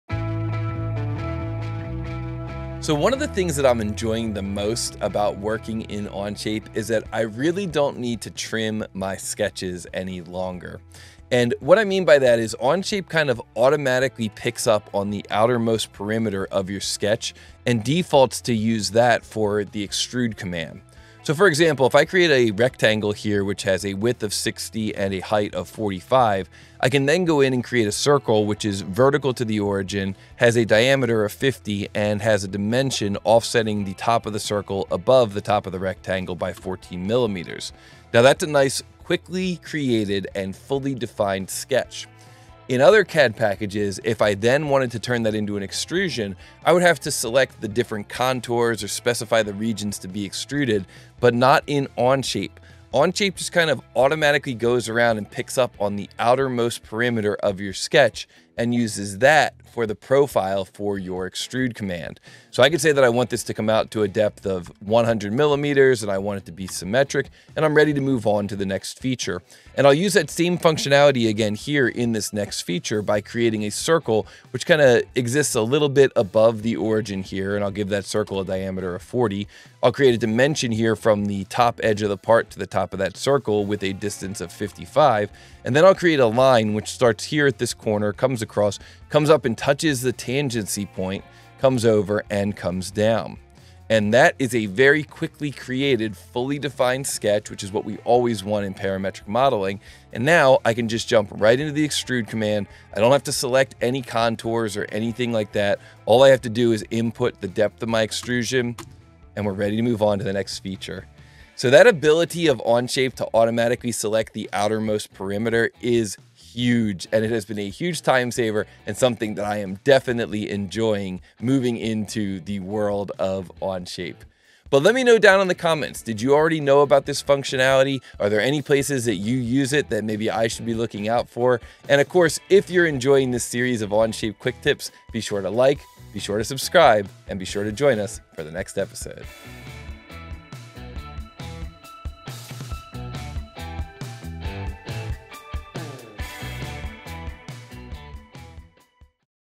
No Need to Trim Sketches in Onshape
Whenever we create sketch geometry in Onshape, we can save time by learning how the Onshape extrude command will default to use the outer most perimeter of the sketch geometry.  This means that in most scenarios, we don't need to TRIM our sketches in Onshape to get them ready to be extruded!